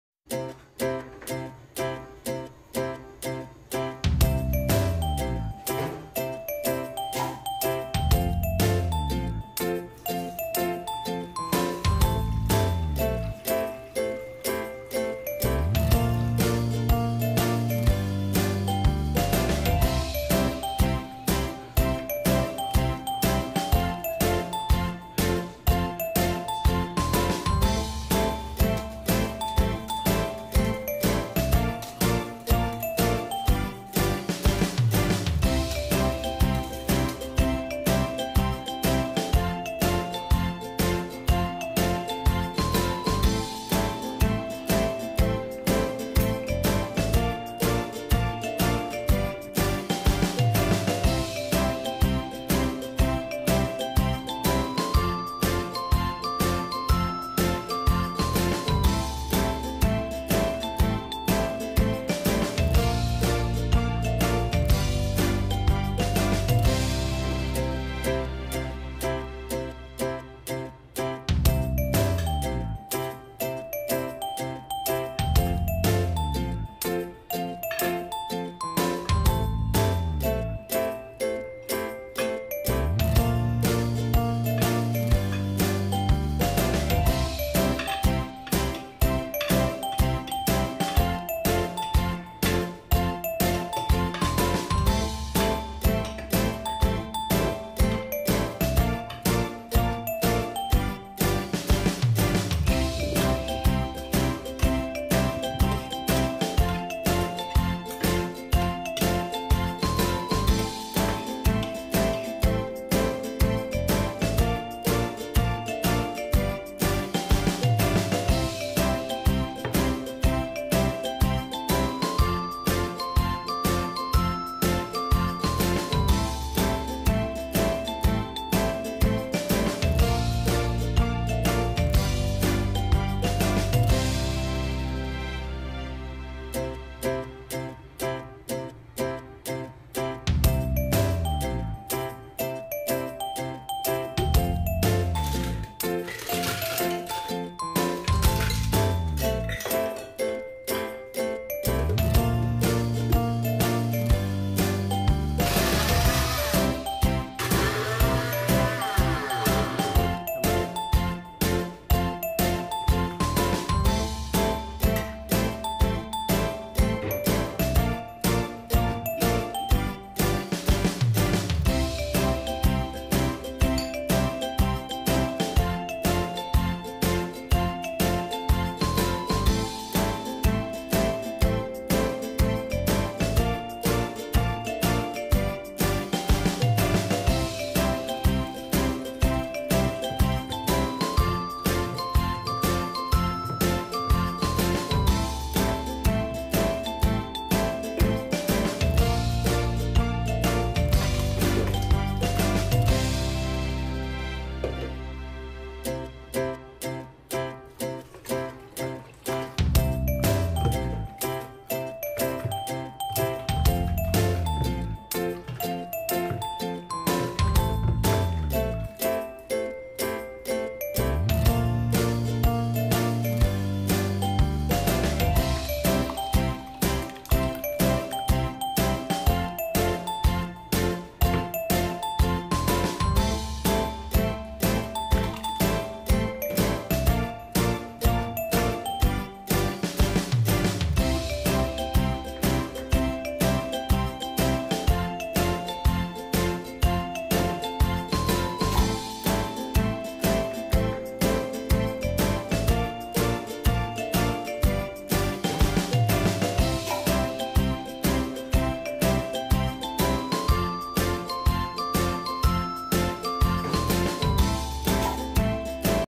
Mango Jelly Drink | Mango Jelly Delight | How to Make Mango Jelly
Ingredients
2 pcs ripe mango
25 grams unflavoued jelly powder
397 ml evaporated milk
210 ml condensed milk
Ice cubes
Cold water